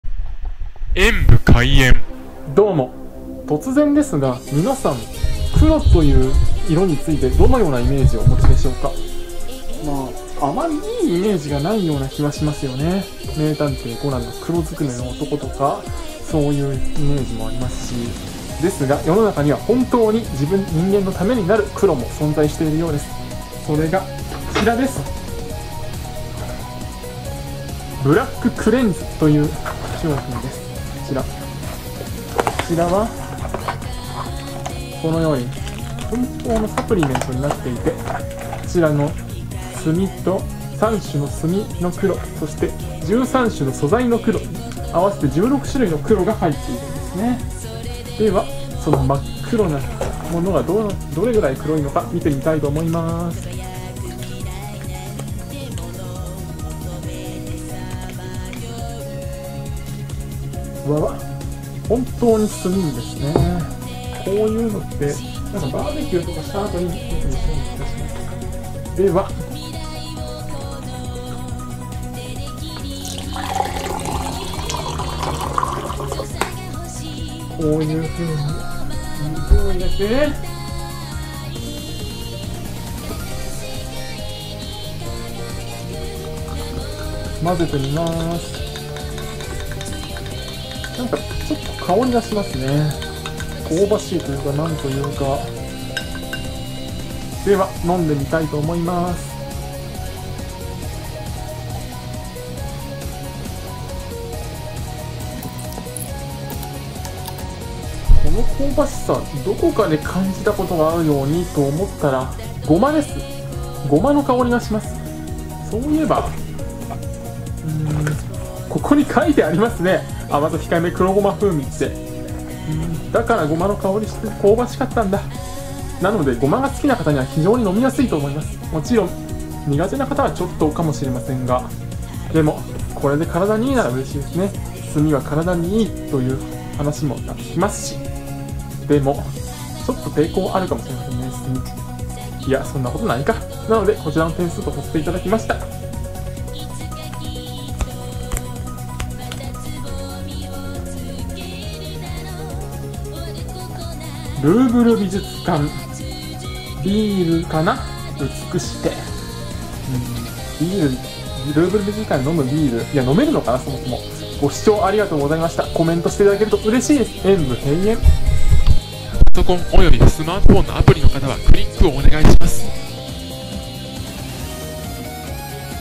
健康に良い黒ドリンクを飲んでみた。これで体を休めよう！！
黒は黒でも健康になれる黒、それが此方！！
体を休めてダイエットにも～
贈り物は此方まで↓
〒153-0042